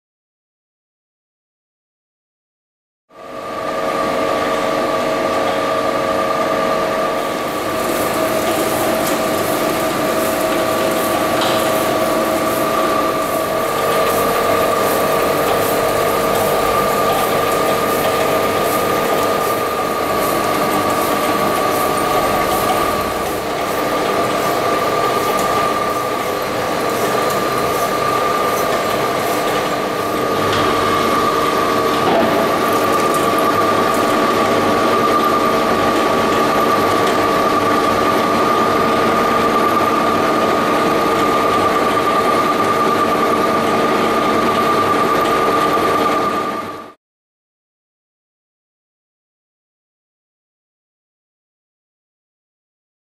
Ward 10 Lot 4
Ward 10 Lathe

General production machining (drilling / turning)

Motor Power:

20 Horse power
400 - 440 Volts
27 Amps
Maximum rpm 580
500 cuts per inch up to 22 cuts per inch feed speeds

Bore 4.25”

Chuck 15” chuck currently on the machine, max size recommended 20”

Bed Length: Chuck to turret 60”

L x W x H: 3950 x 2005 x 1900mm